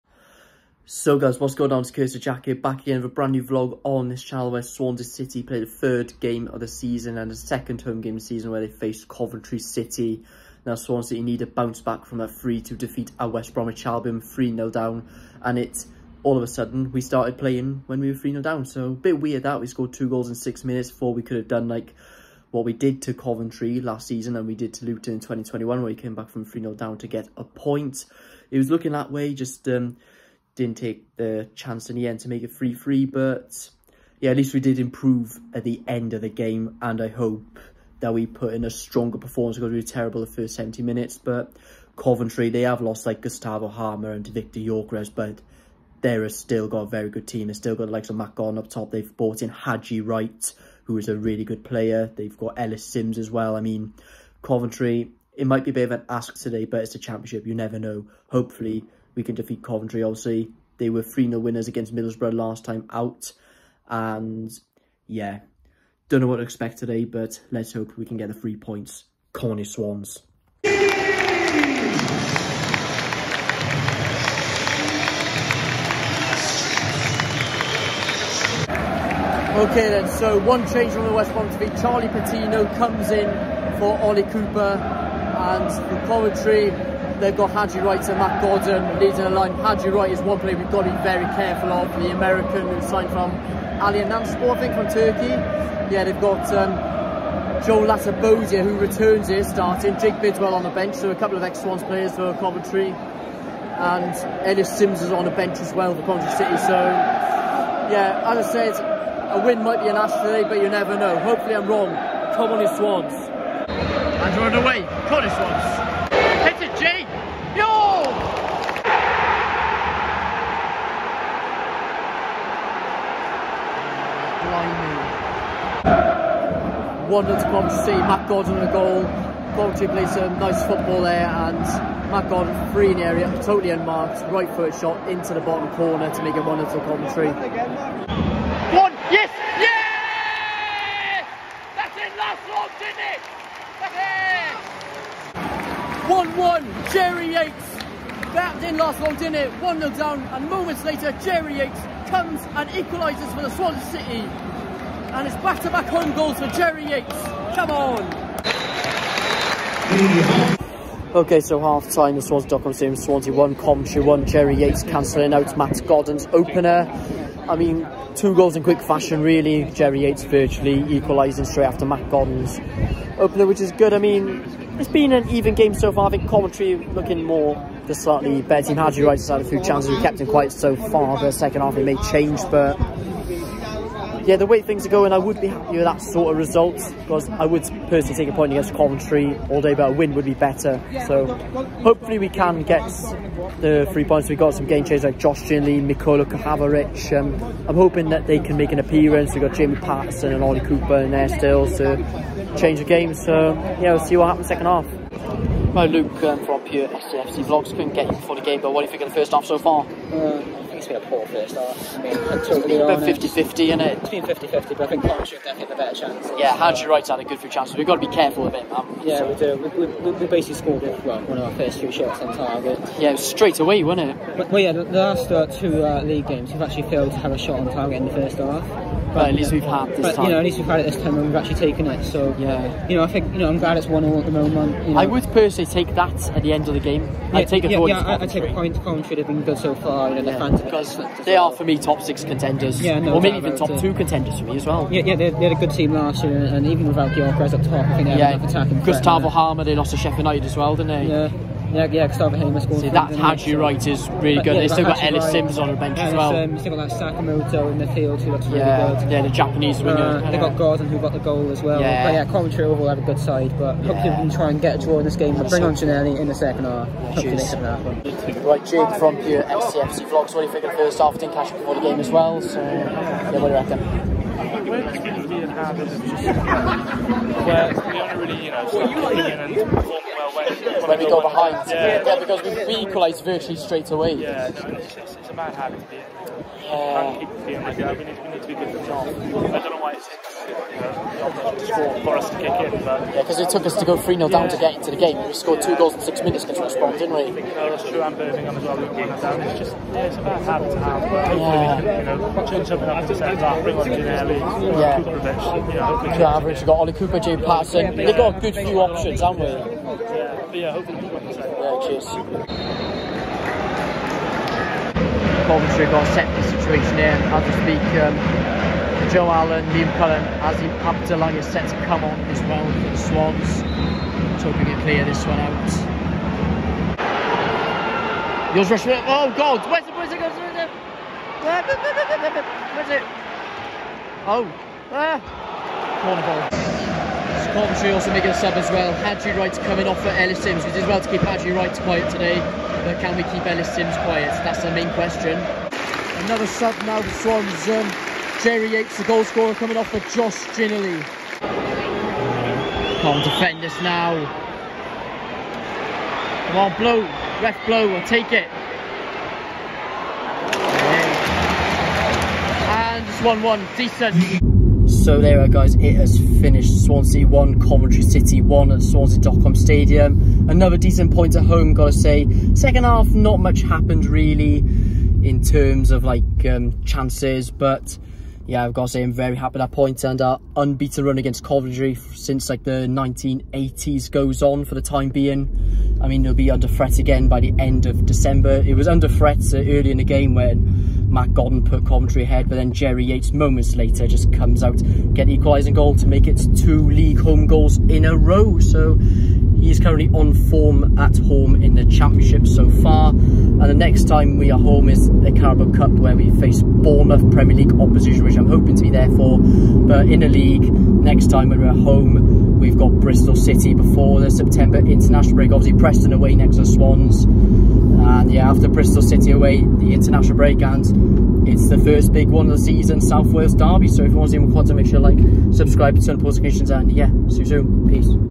SWANSEA CITY 1-1 COVENTRY CITY | YATES SAVES THE SWANS AGAIN! | MATCH VLOG
I vlog our home game against Coventry City in the 23/24 Championship season.